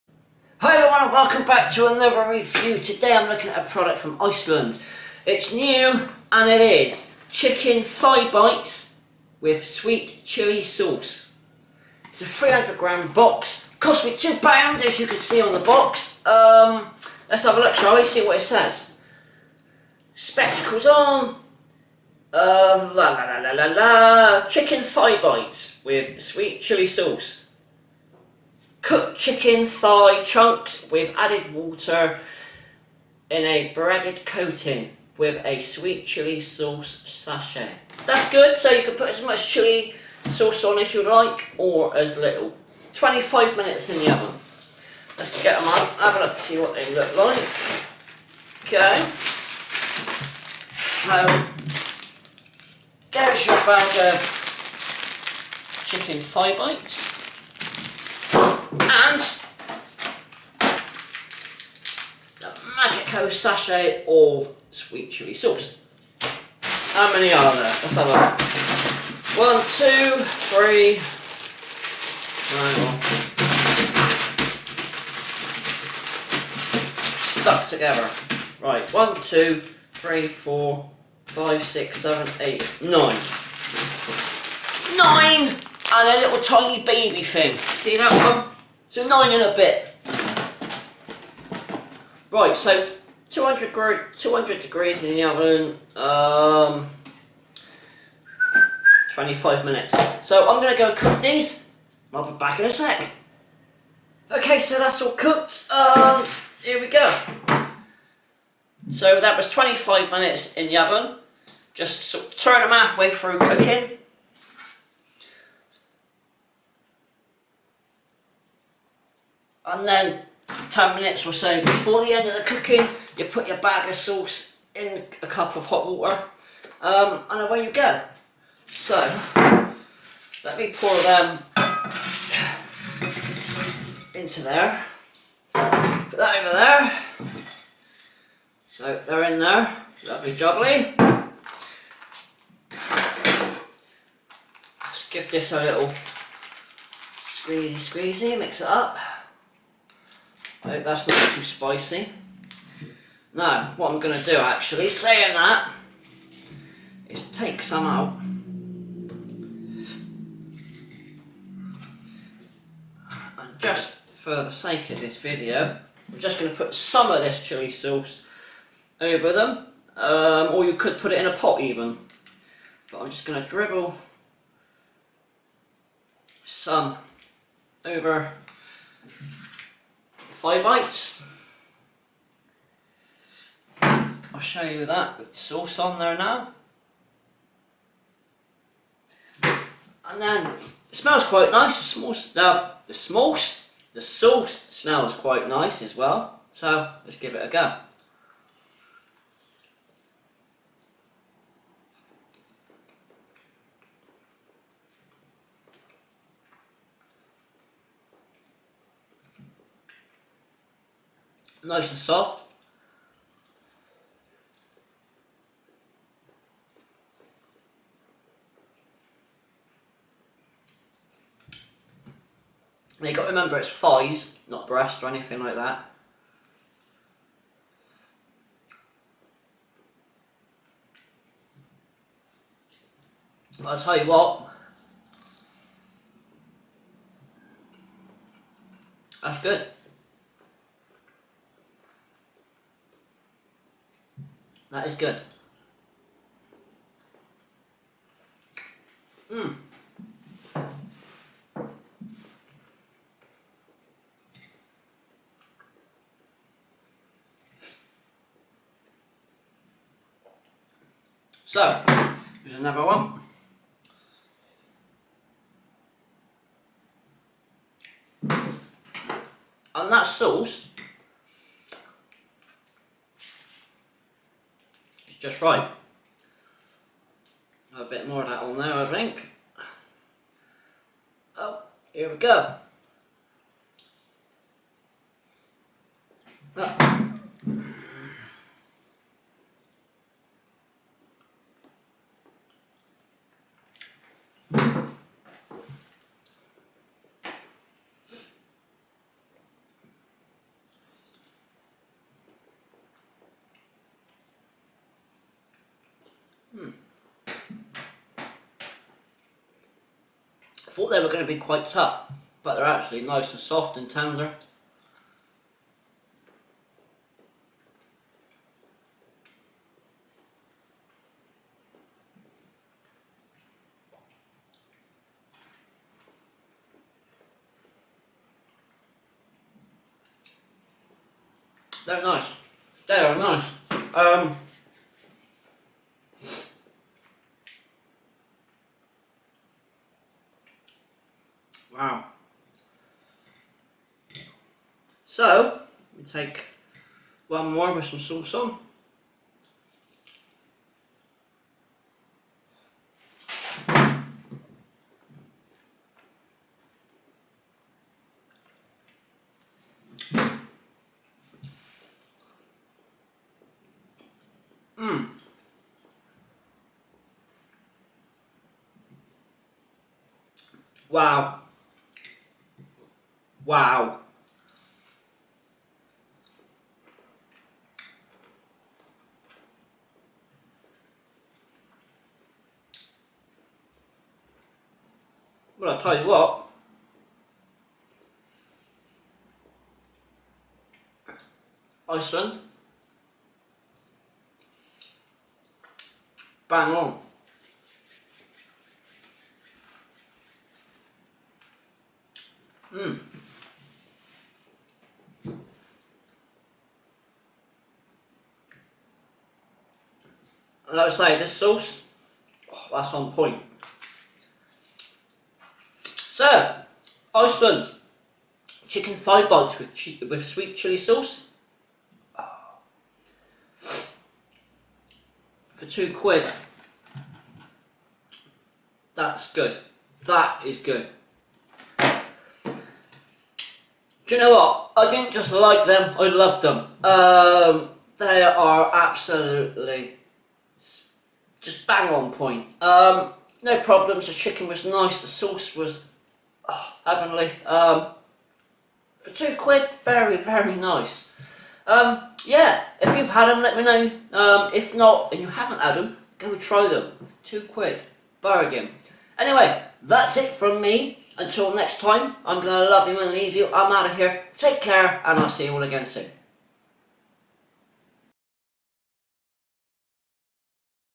Iceland NEW Chicken Thigh Bites with Sweet Chilli Sauce Review
Iceland New Chicken Thigh Bites in a Breaded Coating with a Sweet Chilli Sauce Sachet.

£2 per box (300g)

Thought these looked quite nice, so chucked a box in me trolly, and here we are with another review.